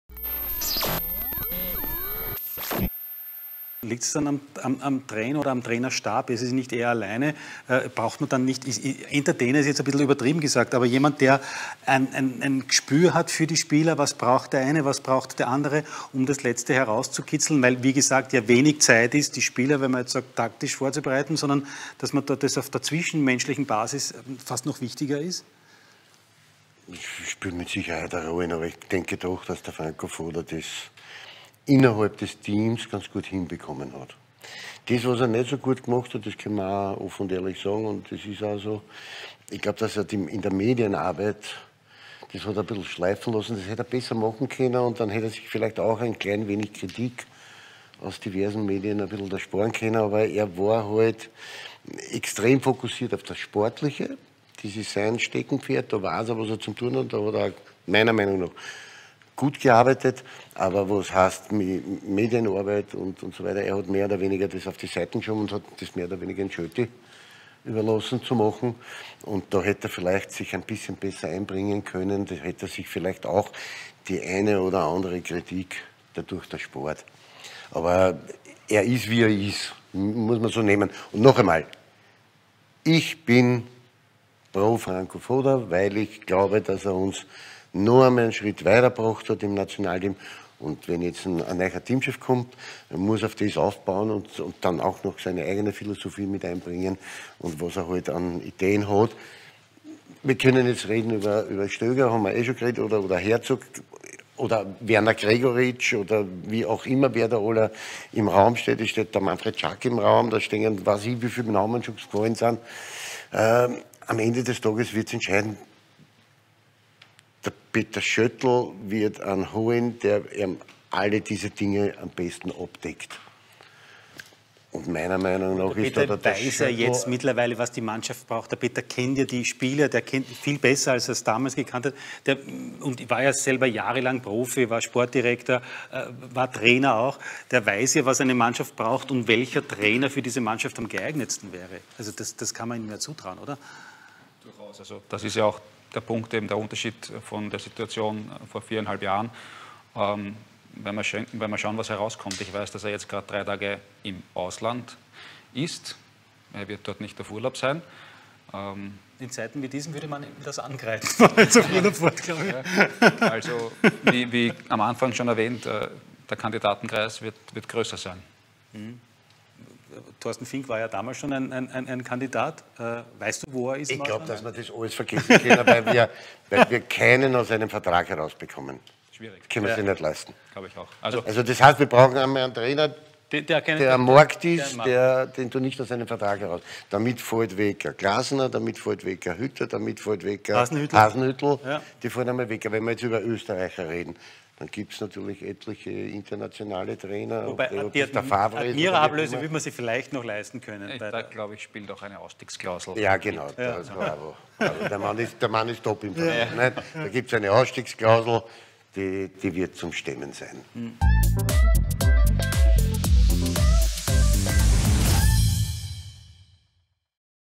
Für Andi Ogris war Franco Fodas größter Fehler der Umgang mit den Medien
„Die Medienarbeit hätte er besser machen können“, meint Austria Legende Andi Ogris. Im Gegensatz zur allgemeinen Meinung stellt er sich in vielen anderen Punkten vor die Arbeit des Deutschen und seinem Trainerteam. Auf der Suche nach seinem Nachfolger soll sich Peter Schöttel laut Andreas Heidenreich vom Kurier gerade im Ausland befinden. Um Glasner, Hütter oder Hasenhüttl soll es dabei aber nicht gehen, denn Ablösesumme werde der ÖFB keine bezahlen.